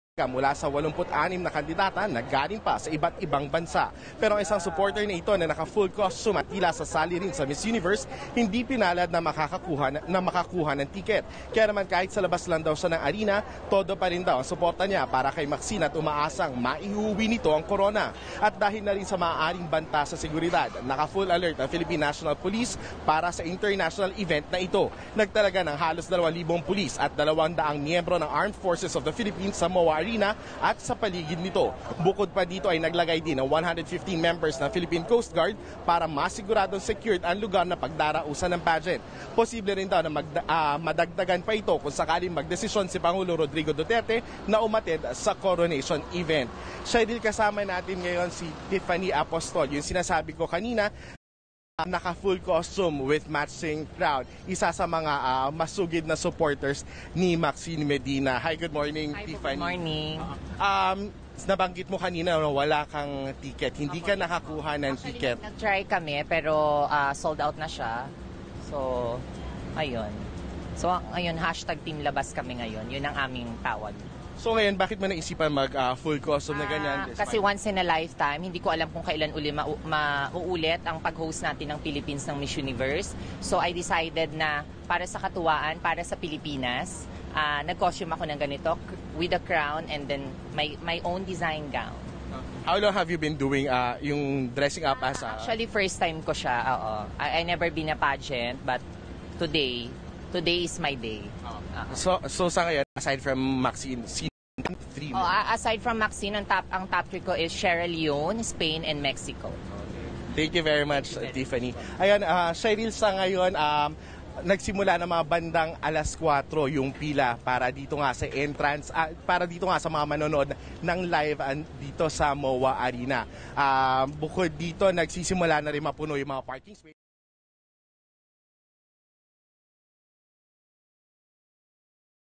Myself | miss universe 2016 Moa Arena thank Newz 5 Aksyon
Thank you 📺 tv5 Newz 5 Aksyon for this interview 😍😘🤩